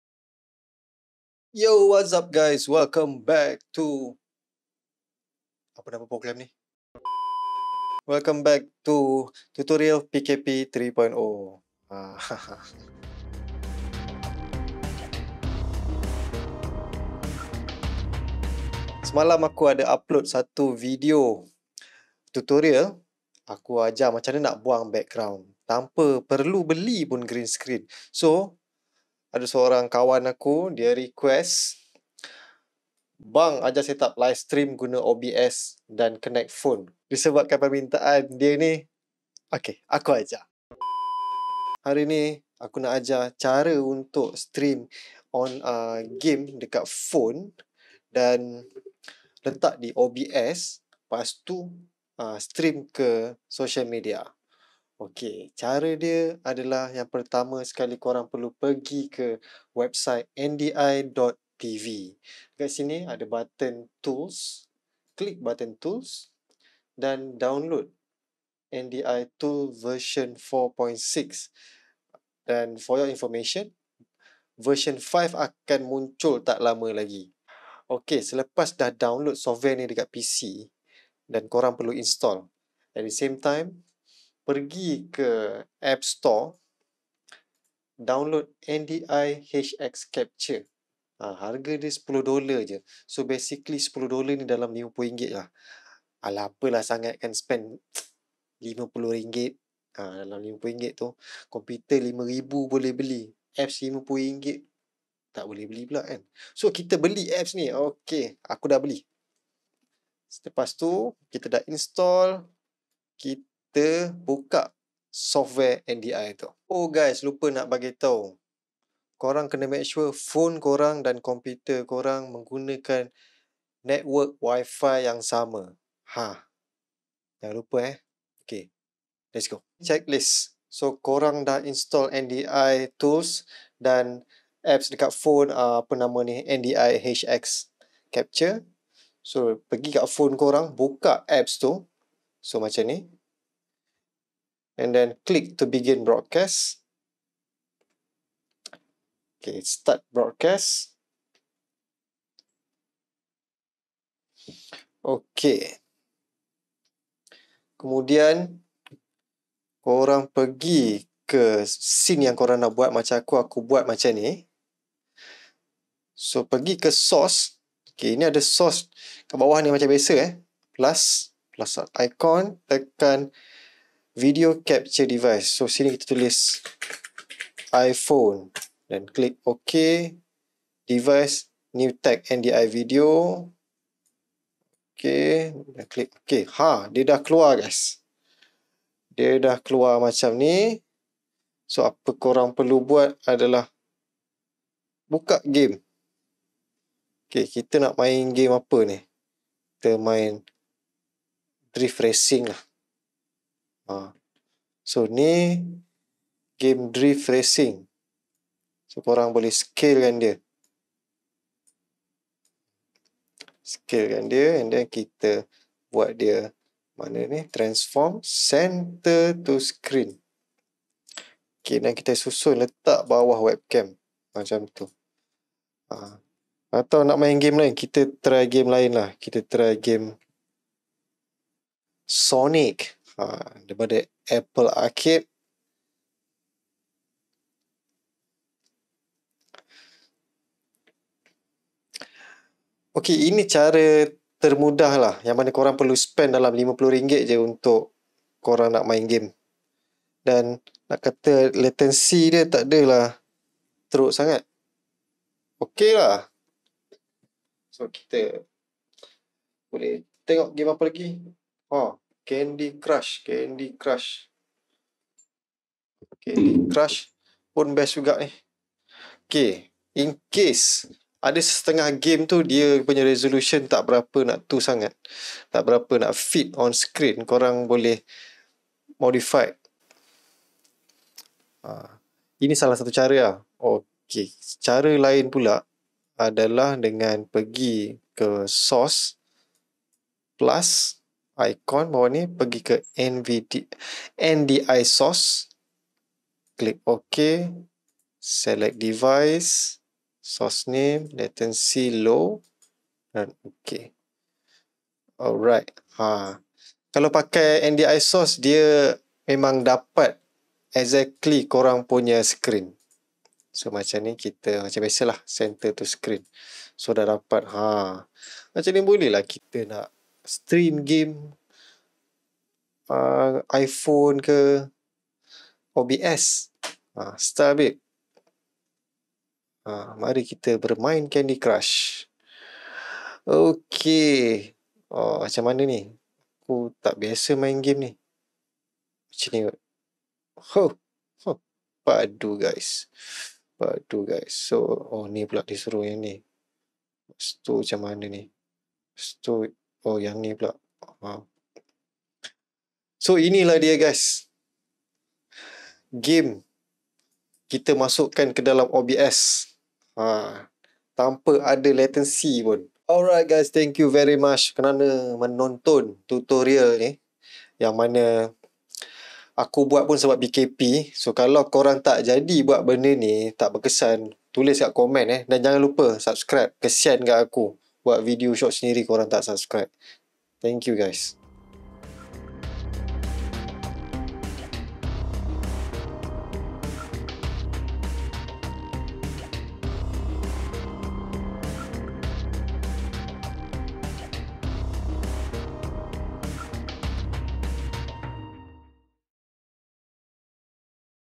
TUTORIAL PKP 3.0 - Cara Live Stream Game HP Dengan Screen Mirror Di OBS FULL HD!!!
Aku ajar cara untuk sambung game COD, PUBG, Candy  Crush dan segala jenis game henpon kat OBS secara wireless. Dannnnn  siaap dapat FULL HD lagi.  Benda senang je geng.